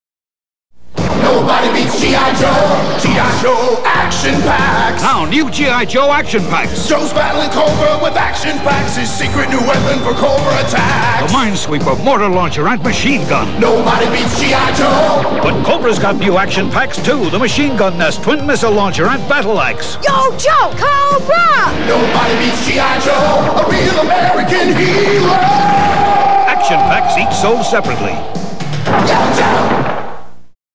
GIJoe 1988 TV Commercial 02: Action Packs '88 - from Griffin Bacal Inc VHS Master
Griffin Bacal Inc. was a global advertising agency founded in 1978 by Tom Griffin and Joe Bacal. Hasbro partnered with Griffin Bacal to create advertising campaigns for G.I.Joe: A Real American Hero, Transformers, and more. This video was digitized from a Griffin Bacal Inc. VHS master dated 3/2/88 and labeled "G.I.Joe 1988 Campaign."